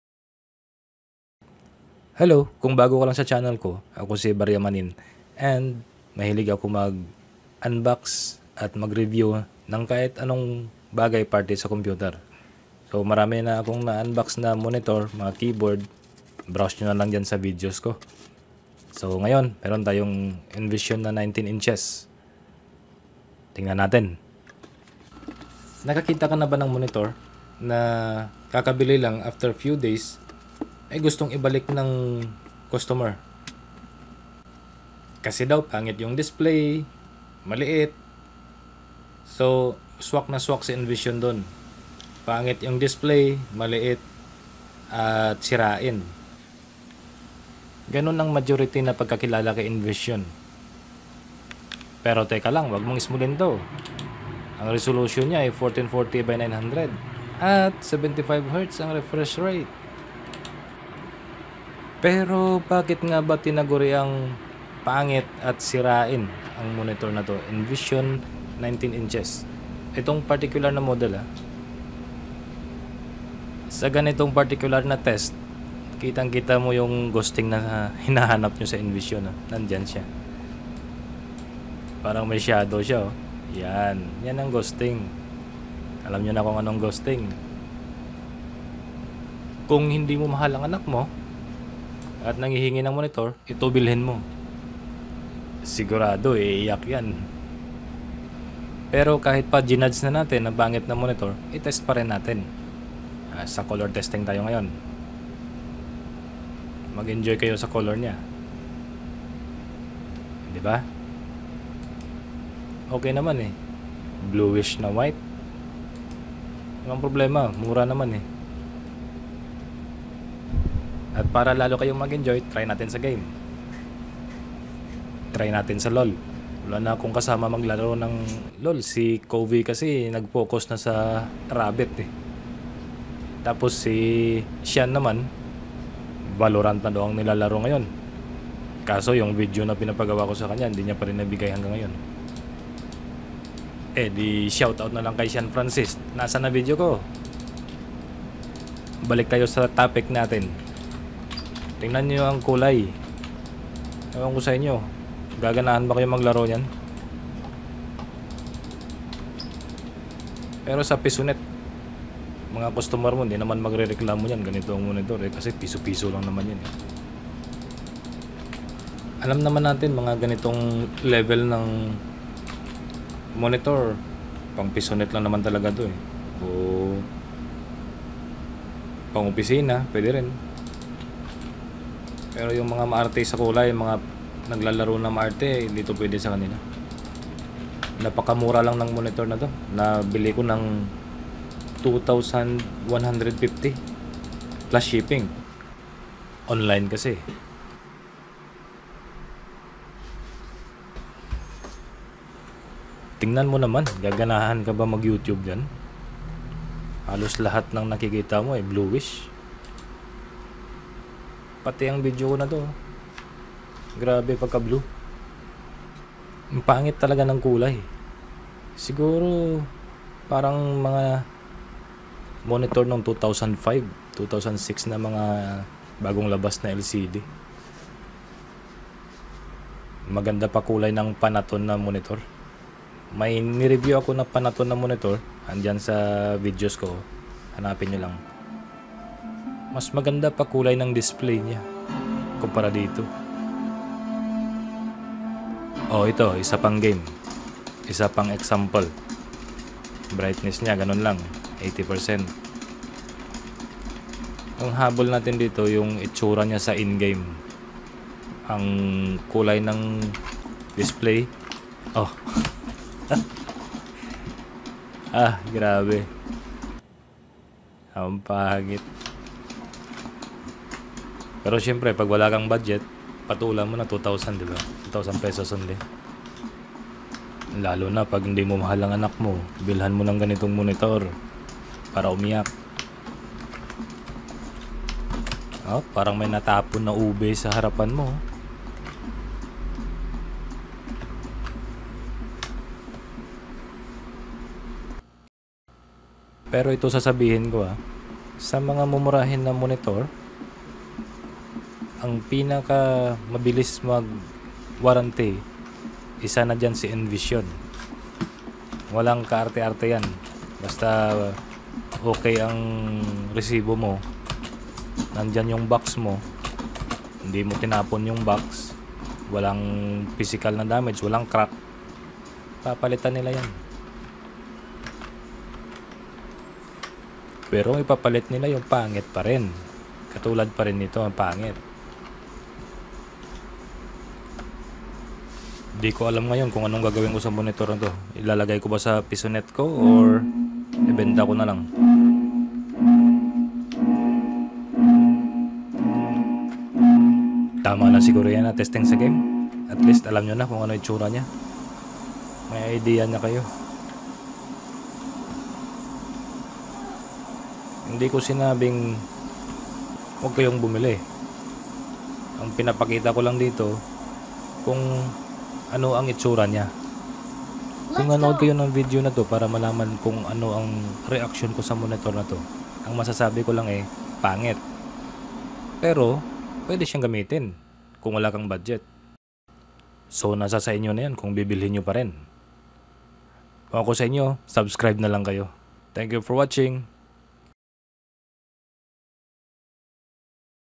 Nvision N190HD 19 inches LED Monitor, 75hz Refresh Rate and 5ms Response Time. 1440*900 Resolution.
Brand: Nvision, Panel Type: TN, Screen Size : 19"
Resolution: 1400×900
Back Light: LED
Response Time: 5ms
Viewing Angle: 80/80/80/80
Active Area: 408.24mmx255.15mm(HxV)
Brightness: 250cd/m2
Refresh Rate: 75hz